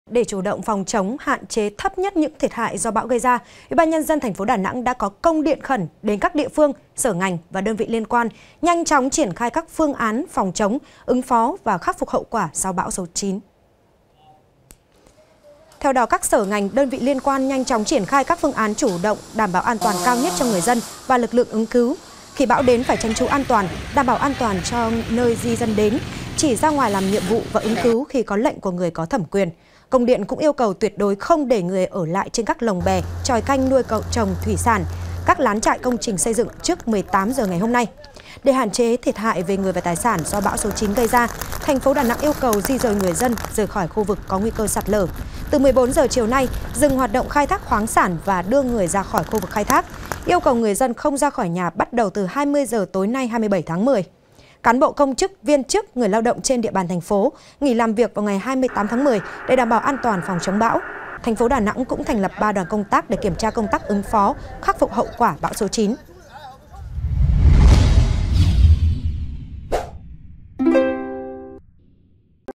Đà Nẵng cấm người dân ra khỏi nhà từ 20 giờ hôm nay| VTC14
VTC14 |Để chủ động phòng chống, hạn chế thấp nhất những thiệt hại do bão gây ra, Ủy ban Nhân dân thành phố Đà Nẵng đã có công điện khẩn đến các địa phương, sở ngành và đơn vị  liên quan nhanh chóng triển khai các  phương án phòng chống, ứng phó và khắc phục hậu quả sao bão số 9.